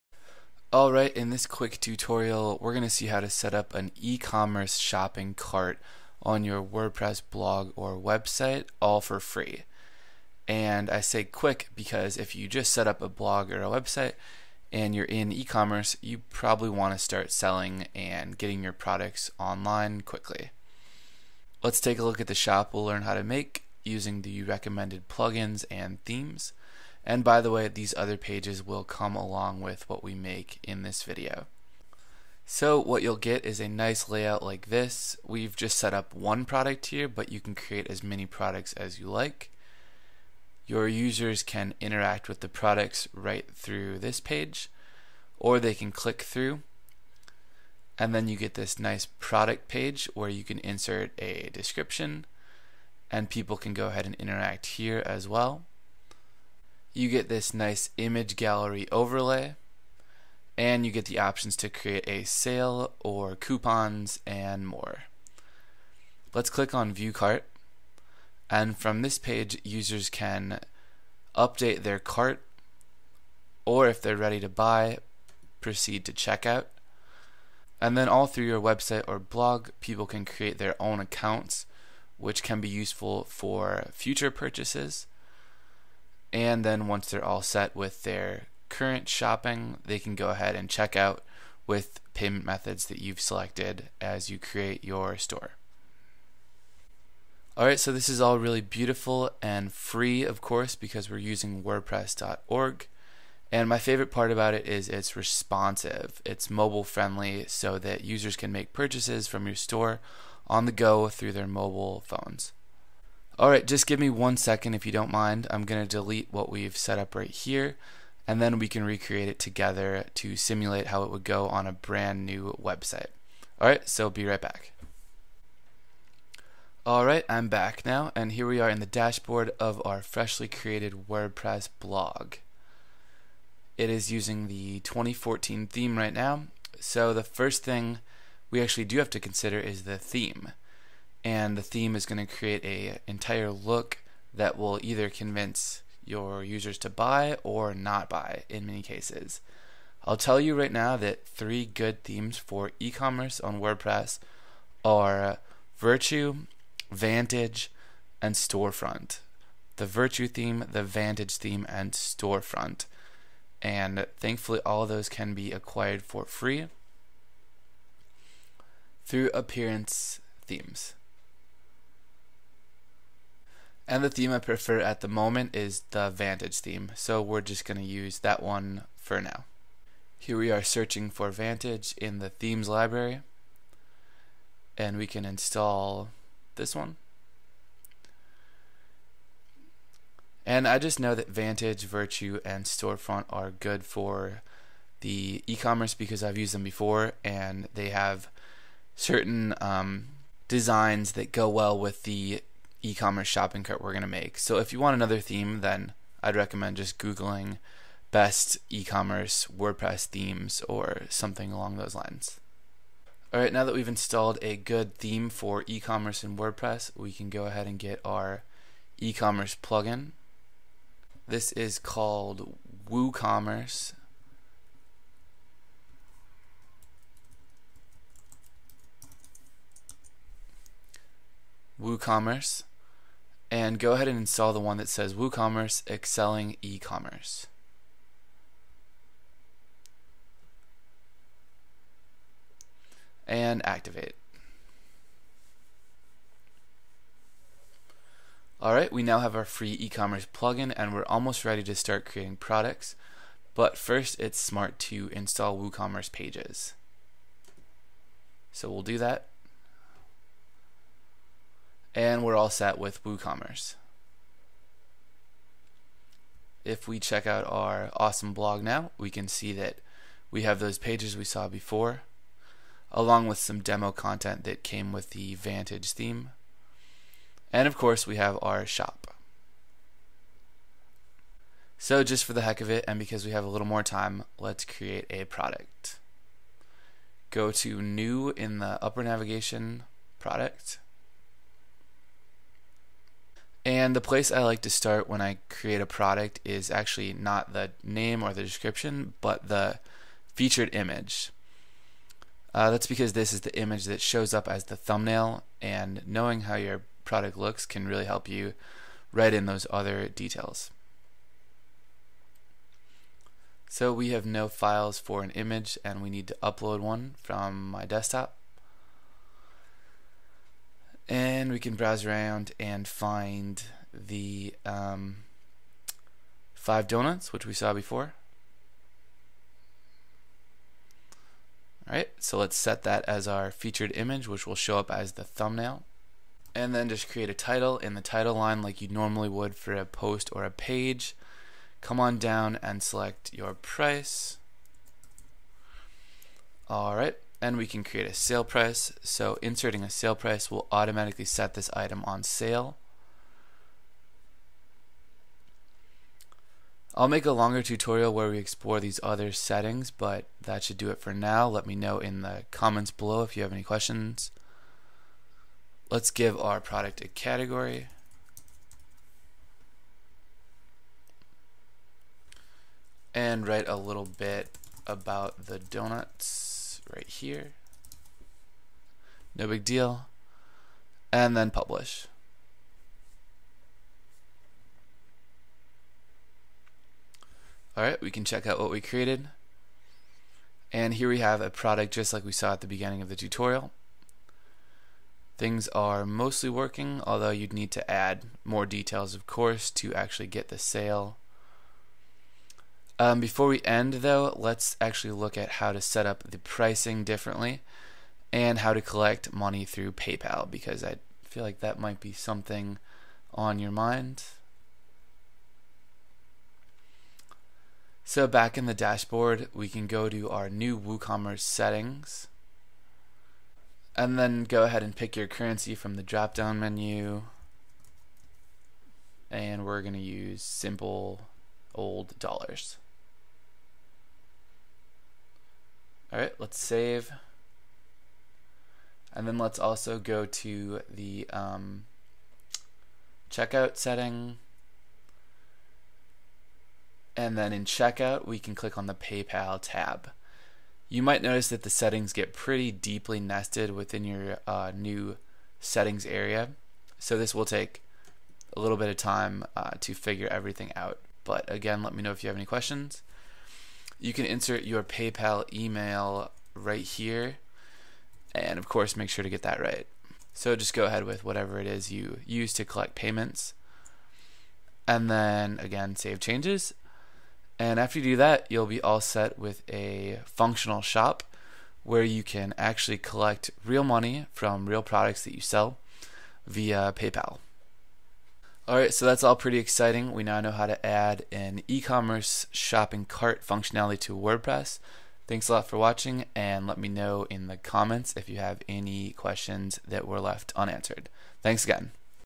How To Create Your Free online Store with Wordpress
How to Create a Simple WordPress Store Online for Free.Build your own free Online shop using wordpress .Sell product online on your free wordpress shop.

Wp free and premium Themes help you create Viral sites.!